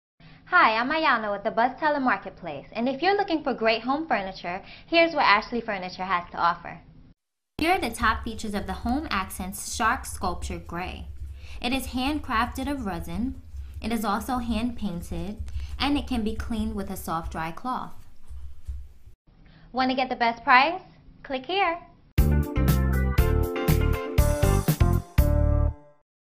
Home Accents Shark Sculpture - Gray | Ashley Furniture Homestore | Home & Garden Features Video
Hi I'm from the Buzztala Marketplace, and here are the top features of the Home Accents Shark Sculpture - Gray | Ashley Furniture Home:
• handcrafted of resin
• hand-painted
• clean with a soft dry cloth
Wanna get the best price? Click here!